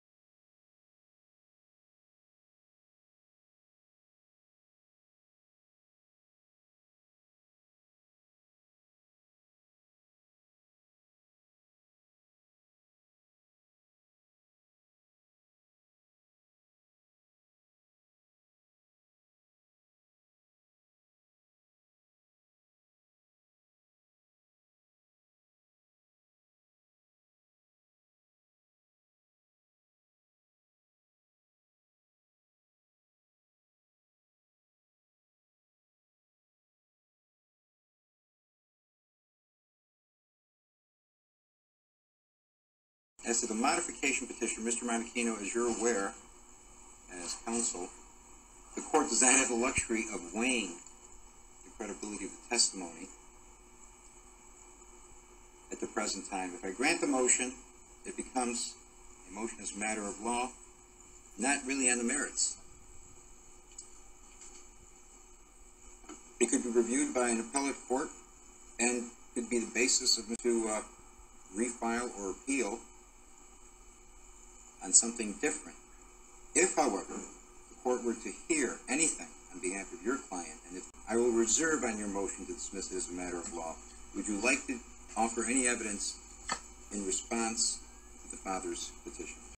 Vote NO for MonachiNO in Broome County Family Court
Sandy MonachiNO was reported for misleading the Broome County Family Court 6 times. He and Judge Pines worked together to prevent a father from being able to appeal the ruling Pines was about to make. Sandy Monachino is everything that is wrong with the family court today.